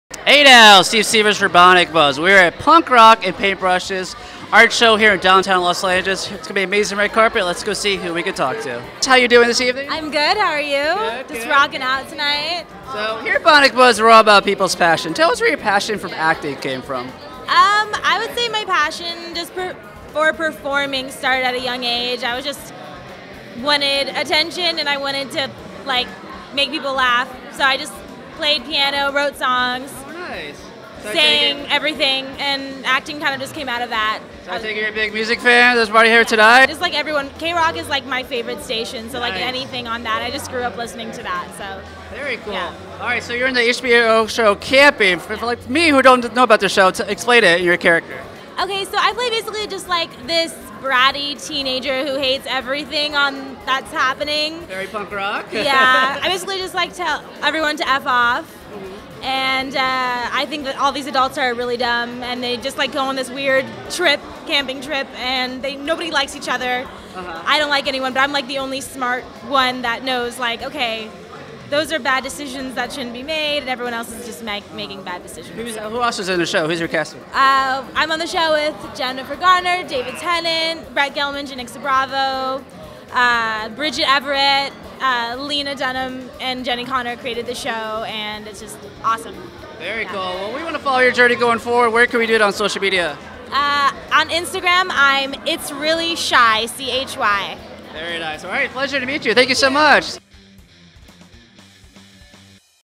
Actress Cheyenne Haynes Interview at Punk Rock & Paintbrushes Arts Show
Bionic Buzz® got to cover Punk Rock & Paintbrushes Holiday Art Show at Innerspace Gallery in Downtown Los Angeles. The event celebrated the fine art of creatives who share a passion for both music and art. The event also benefited the The Sidewalk Project, a Los Angeles non-profit whose initiative is to enrich the lives of the Los Angeles’ houseless community.

At the event we got to interview actress Cheyenne Haynes from HBO’s Camping.